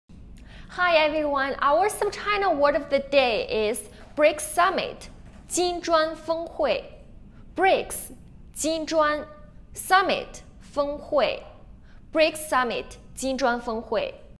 SupChina word of the day Episode 48
Hi, everyone. Our SupChina word of the day is BRICS summit 金砖峰会 (jīnzhuānfēnghuì).